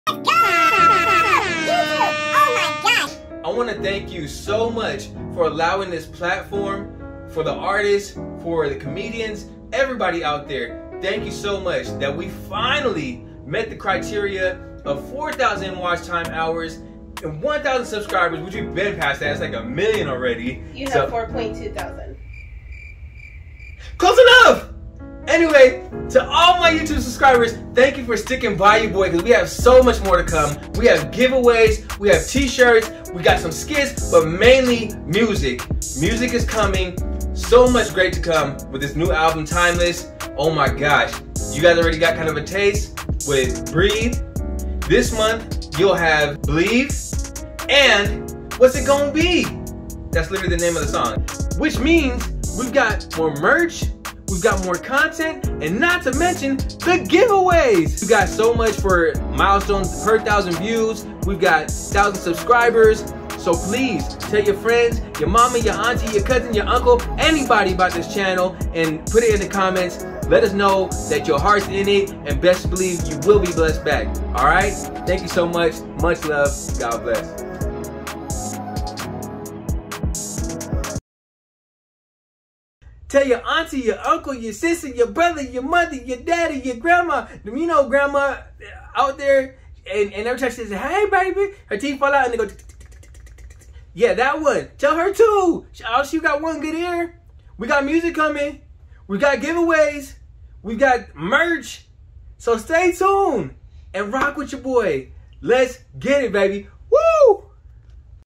Officially a Youtube Affiliate Lets Goooooo!
So excited to bring you new content 2 new songs dropping this month and much more to come since I am now a partner of Youtube such an accomplishment.